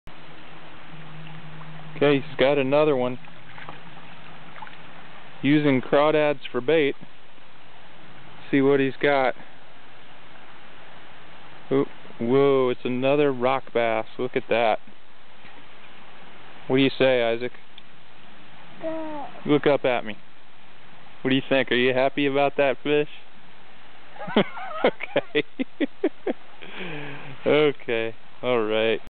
Another Rock Bass
Using live crawdad for bait, Isaac catches yet another Rock Bass on the East Fork of the Little Miami River, Clermont County, Ohio. June 18, 2011